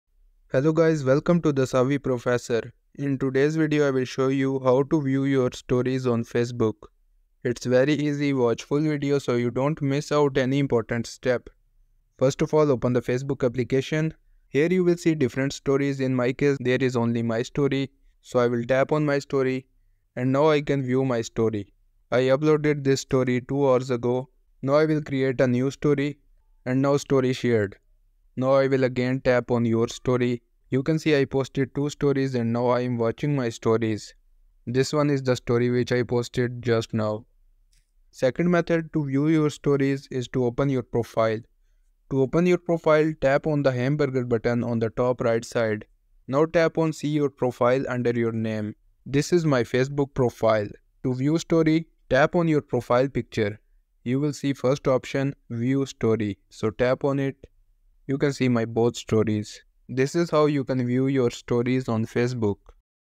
How to View Your Stories on Facebook App (How to View Your Own Story on Facebook)
How to View Your Stories on Facebook (How to View Your Own Story on Facebook).In this tutorial, you will learn how to view your stories on Facebook.